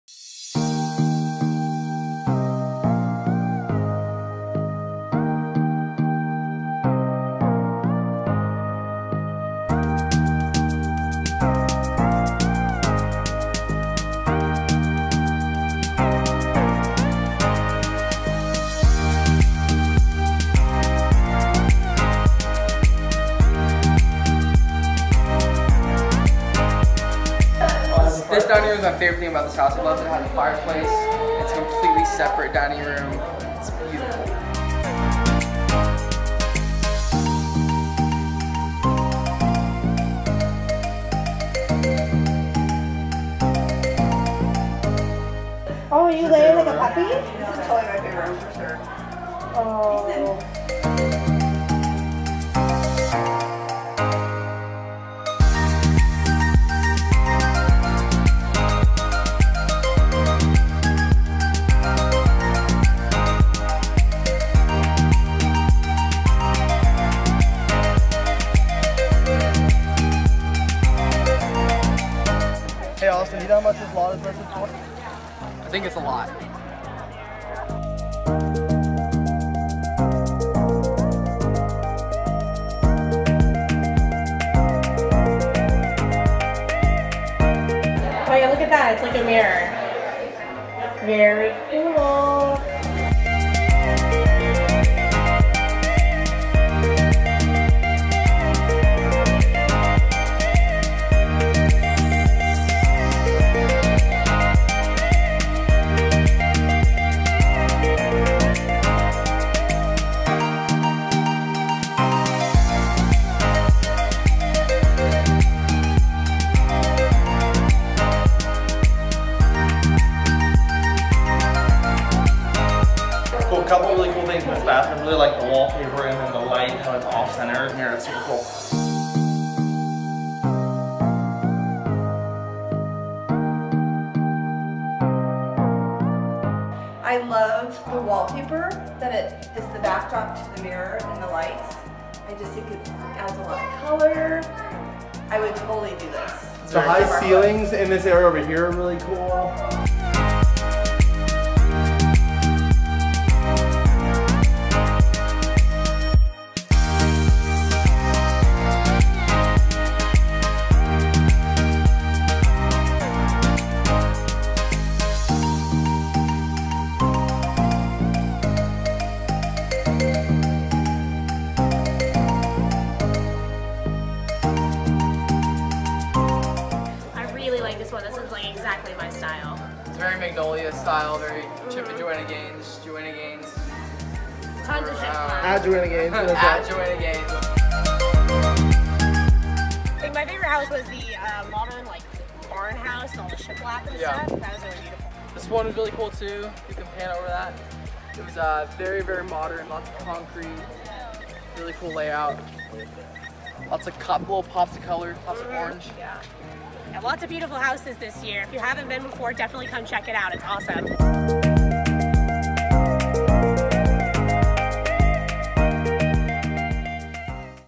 Portland Street of Dreams | Theory Vlog 36
This week the Theory Team headed down to Portland to attend Street of Dreams! It was an awesome time seeing some INCREDIBLE homes and talking with other pros in the Real Estate Industry!